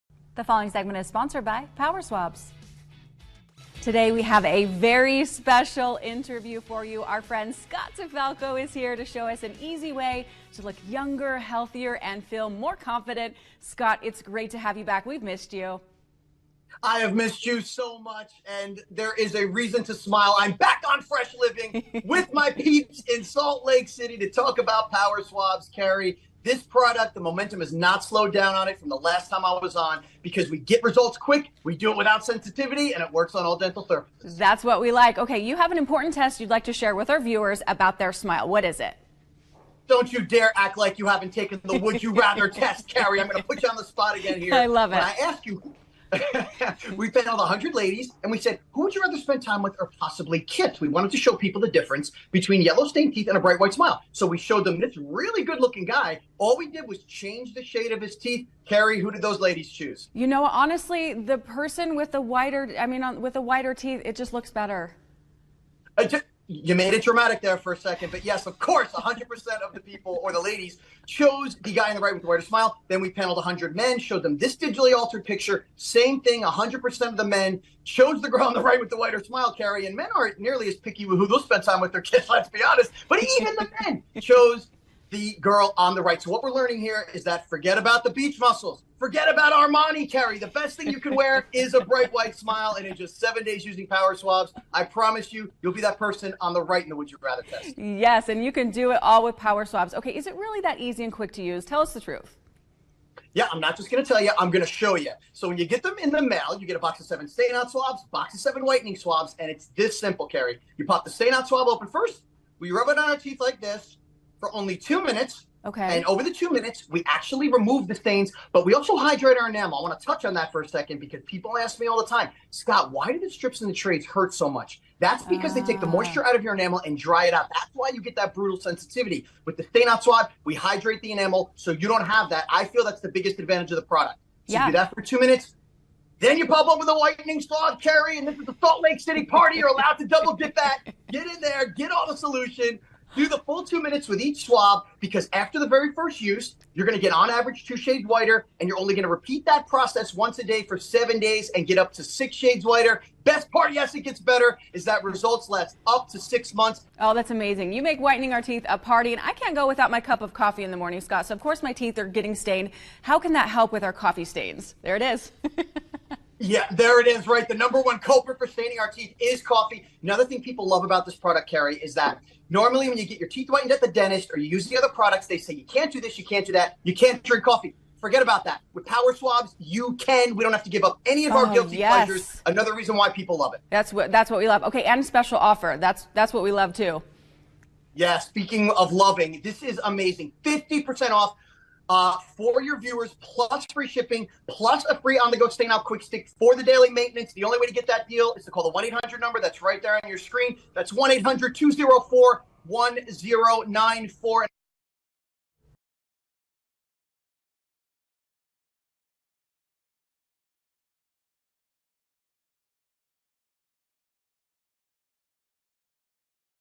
How to remove stains on your teeth
Are you tired of having yellow teeth?

Kari spoke to Scott DeFalco about Power Swabs.